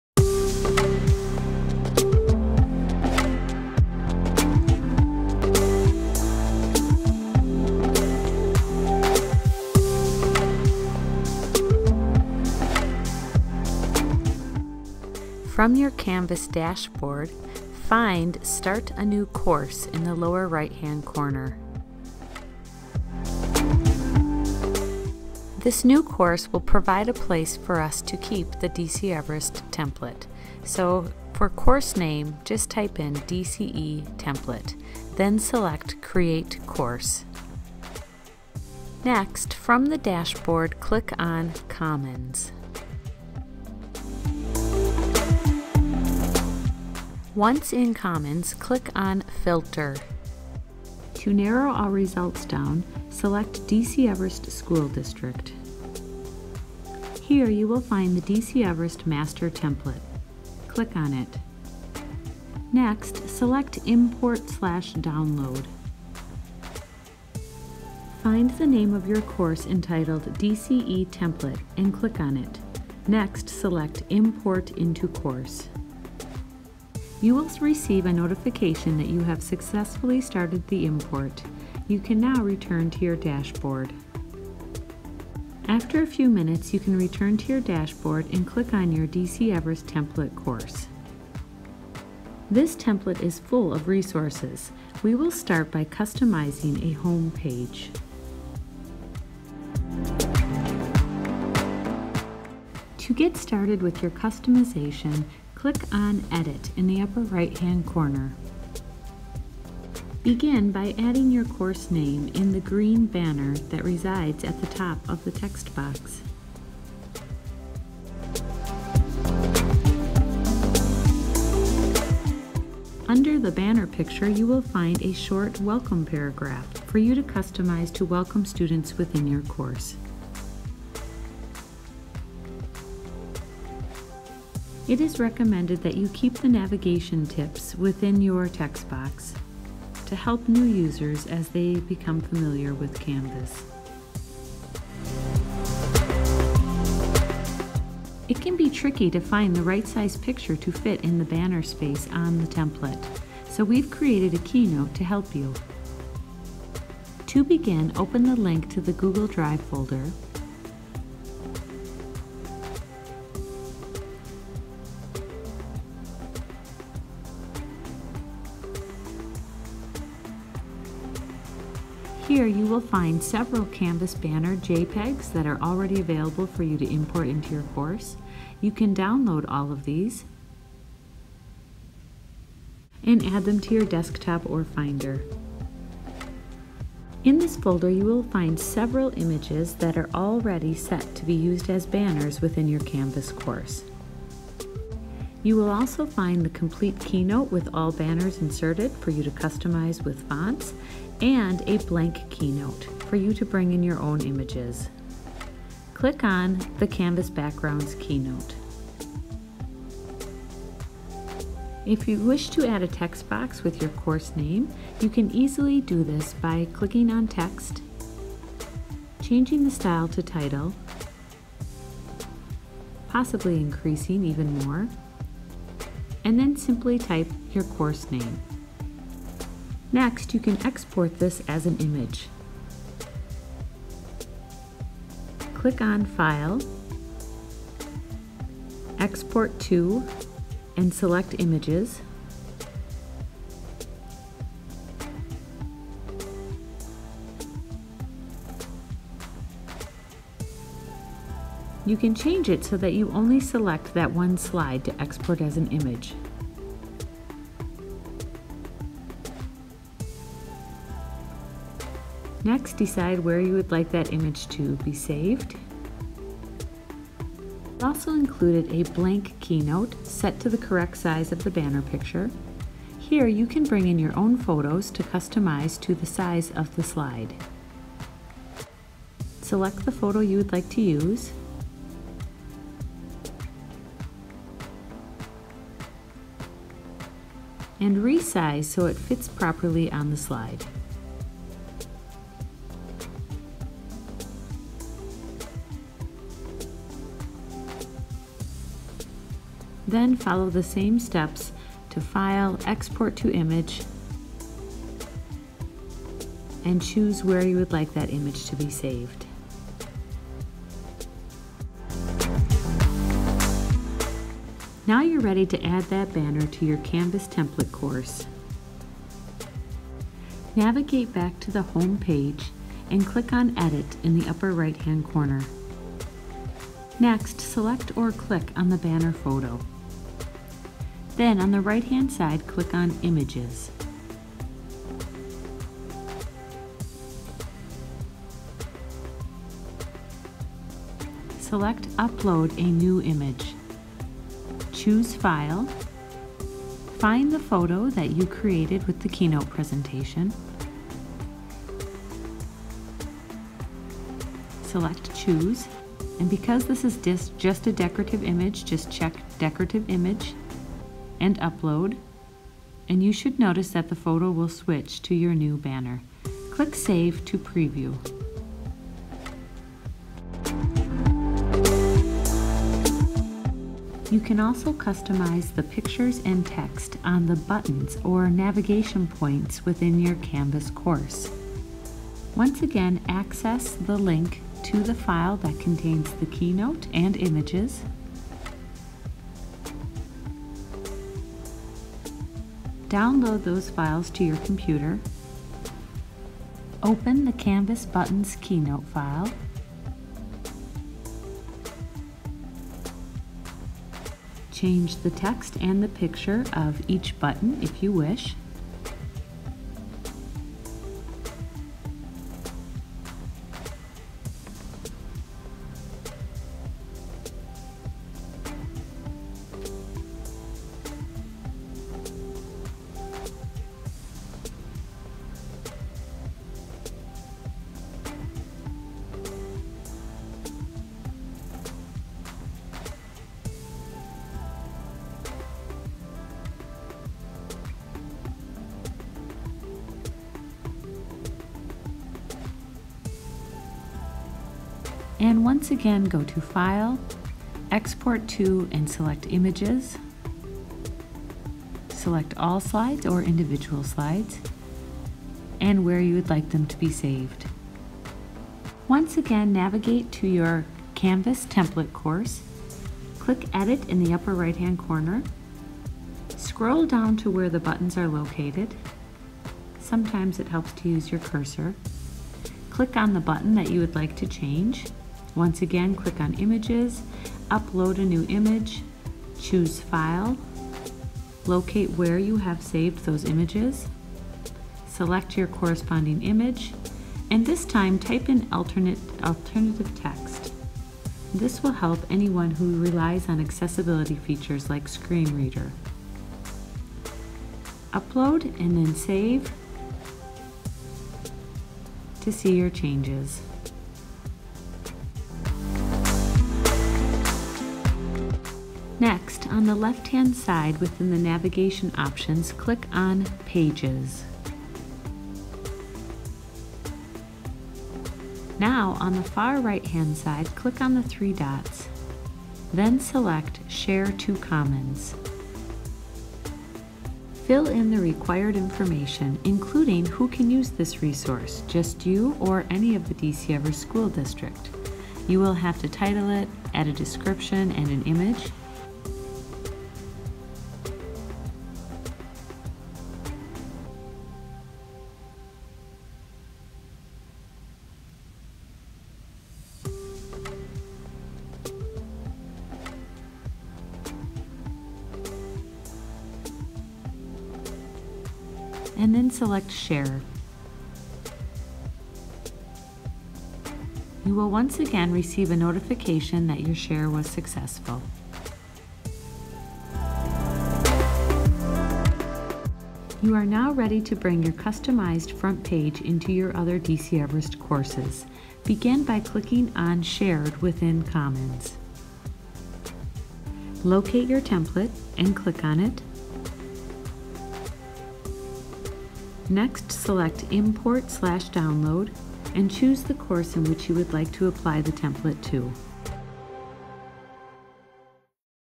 D.C. Everest School District - Customizing Canvas
How to use the template and customize the home page.